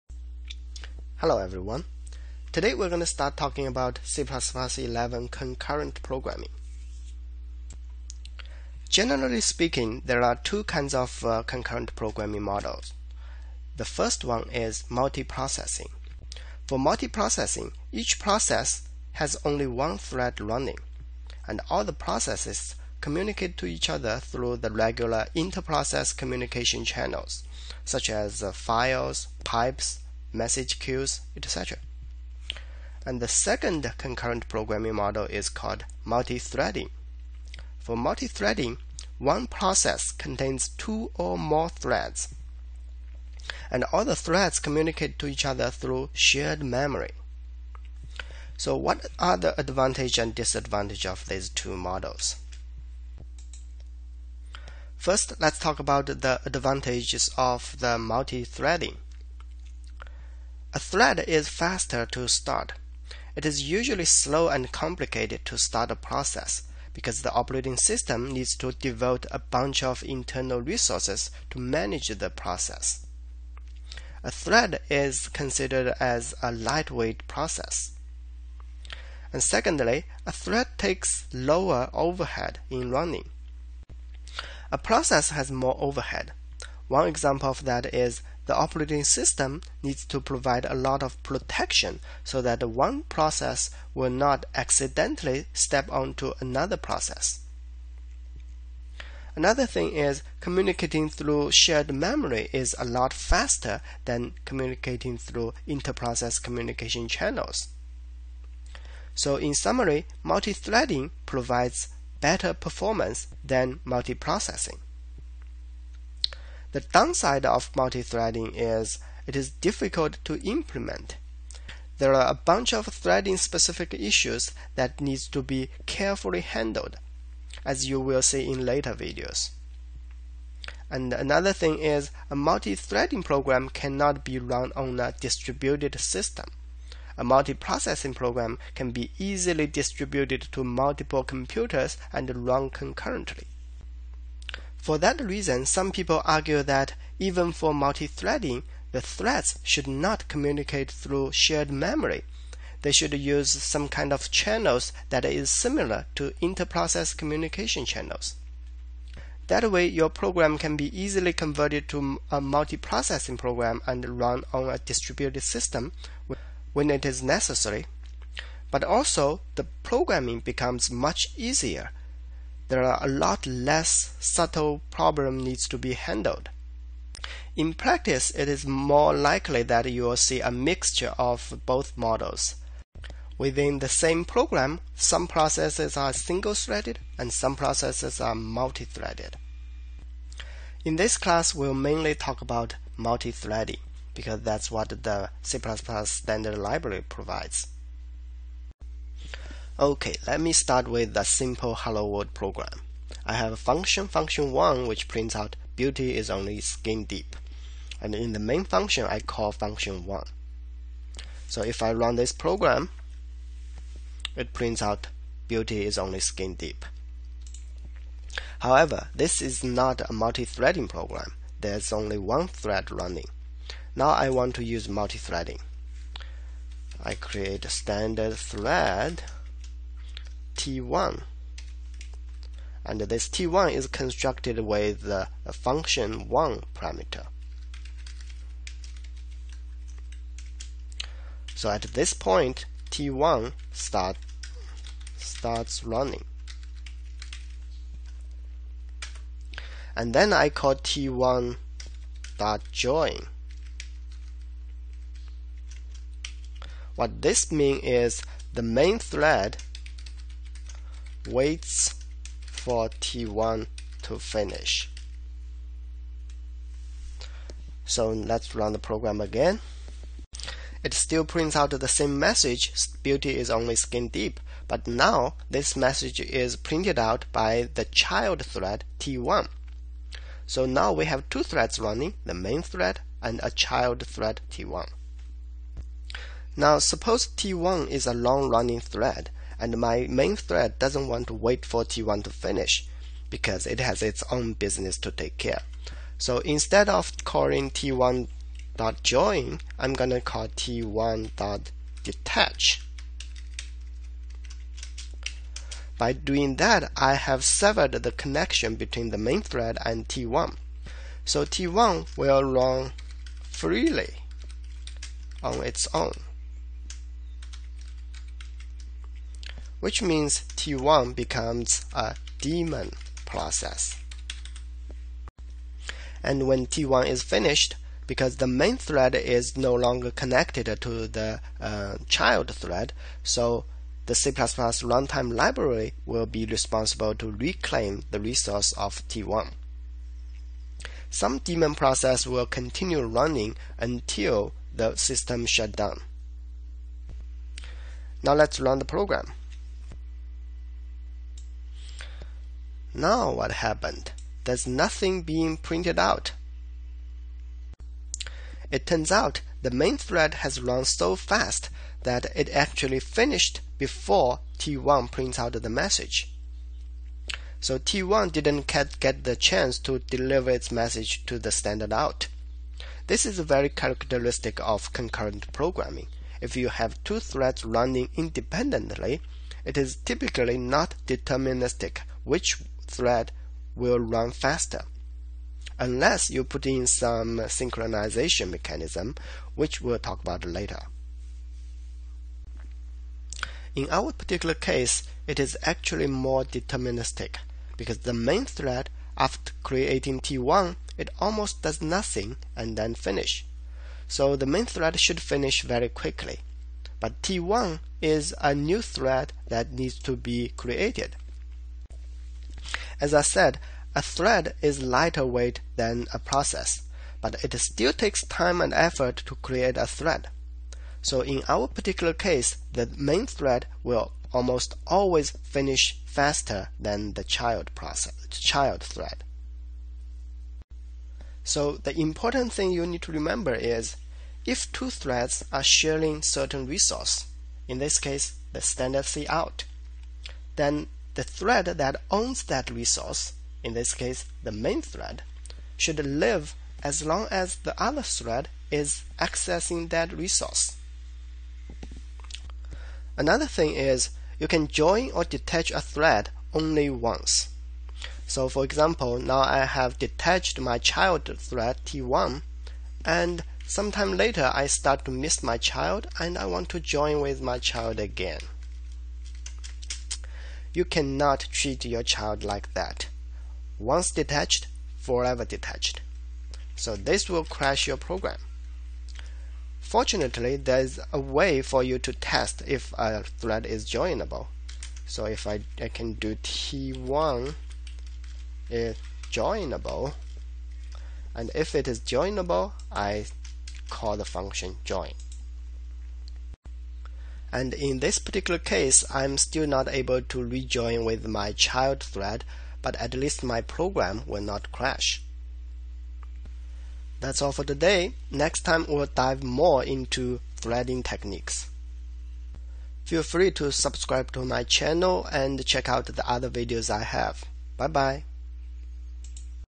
C++ Threading #1: Introduction
This is the first video that talks about concurrent programming in C++ 11.  It gives an introduction of multiprocessing and multithreading, and then gives a first example of multithreading.

Notes can be downloaded from: boqian.weebly.com